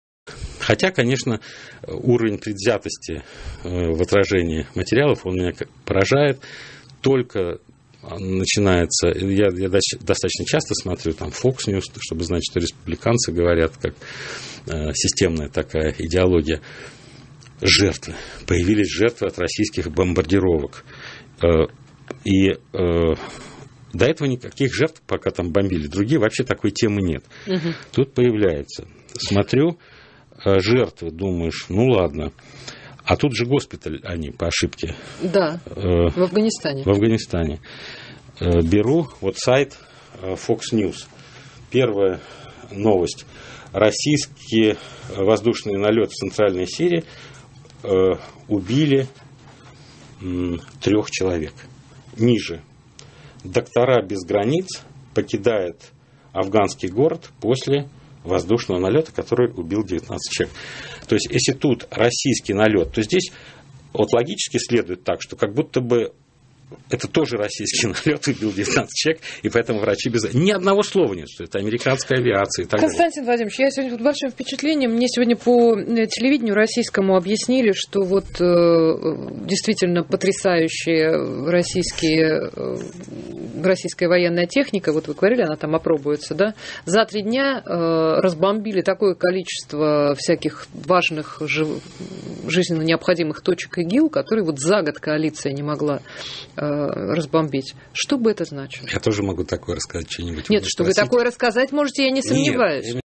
Эховская мразь затыкает рот Ремчукову
Ремчуков рассказывает о своих впечатлениях от лжи в американских сми, и тут эховская сучка его абсолютно нагло перебивает и переводит тему на обычное эховское дерьмо..